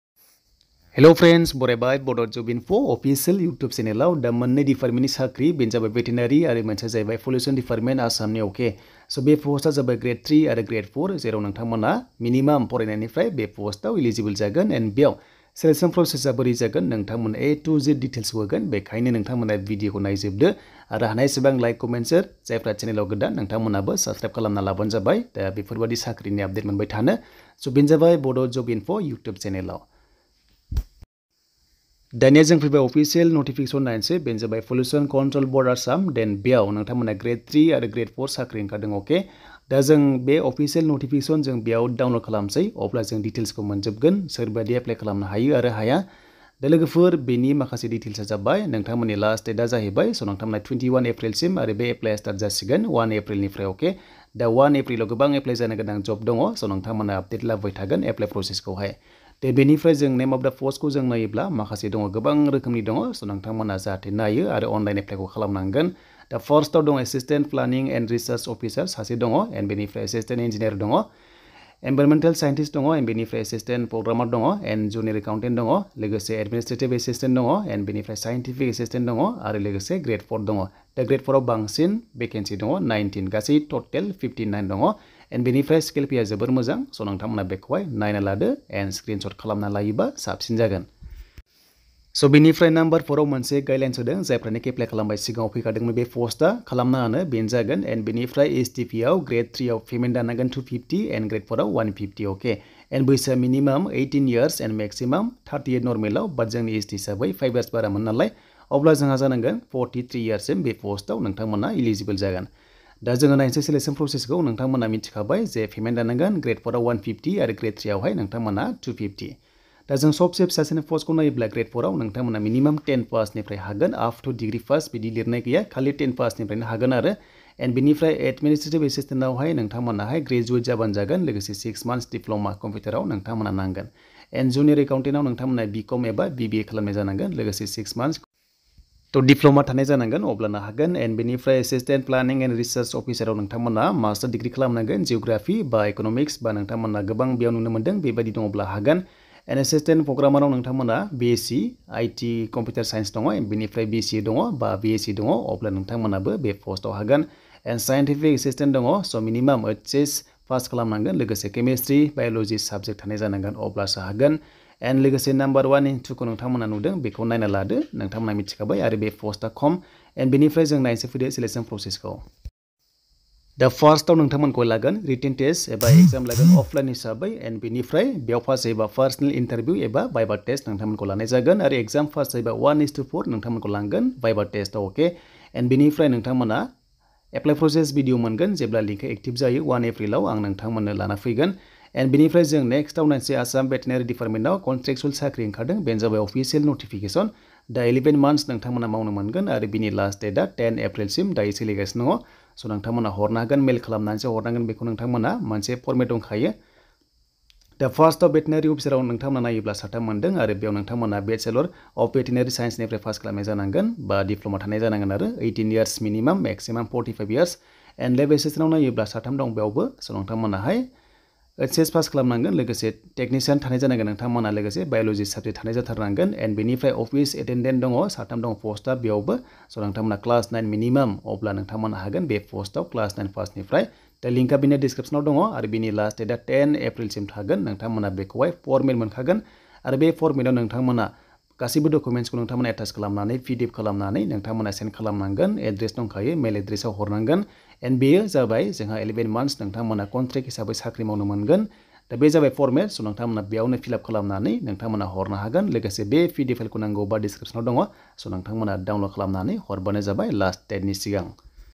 गोदान साख्रि ओंखारदों Assam Pollution Dept & Veterinary आव Gr.4 Gr.3 Male Female ||Bodo Job Info
Assam Pollution Control Board Dept Recruitment Big Update Grade III and IV Level Post

Full Advertisement

Education Qualification Minimum 10th Pass  Level
Exam Fee: 150 for Grade IV and 250 for Grade III.

Assam Veterinary Recruitment for the post of Grade III and IV Big Update 2023

BODO JOBS UPDATE, Bodo Job Info, Bodo Jobs Update Bodo, Bodo Information Jobs, Bodo Job Information Jobs.